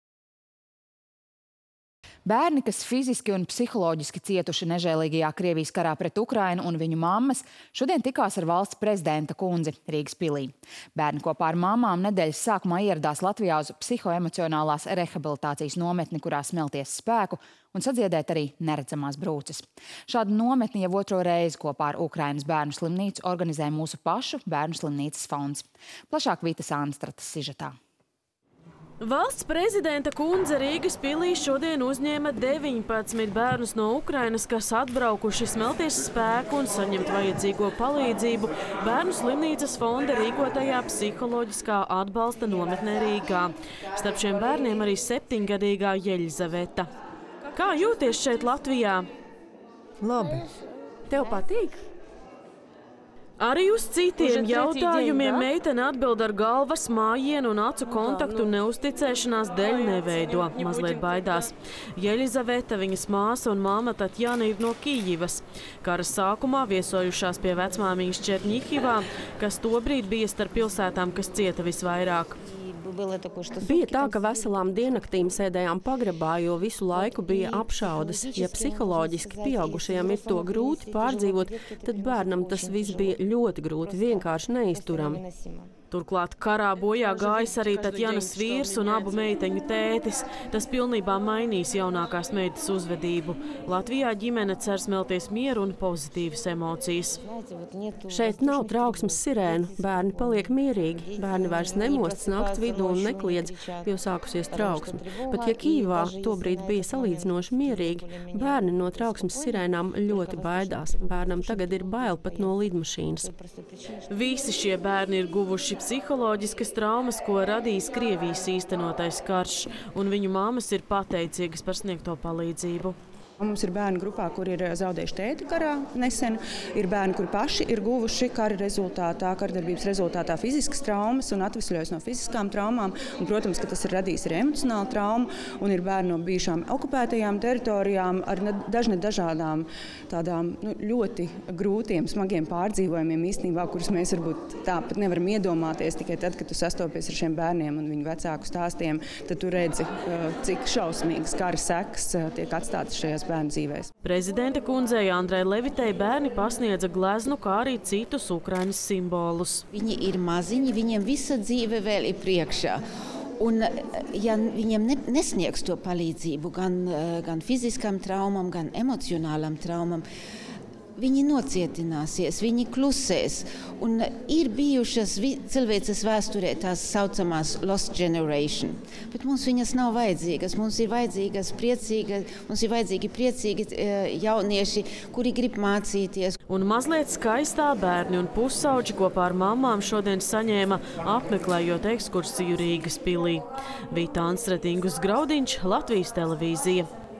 Ukrainas Bērnu slimnīcas pacienti apmeklē Rīgas pili
Bērni, kas fiziski un psiholoģiski cietuši nežēlīgajā Krievijas karā pret Ukrainu, un viņu mammas šodien tikās ar Valsts prezidenta kundzi Rīgas pilī. Bērni kopā ar mammām nedēļas sākumā ieradās Latvijā uz psihoemocionālās rehabilitācijas nometni, kurā smelties spēku un sadziedēt arī neredzamās brūces. Šādu nometni jau otro reizi kopā ar Ukrainas Bērnu slimnīcu organizē mūsu pašu Bērnu slimnīcas fonds. Plašāk – Vitas Anstrates sižetā.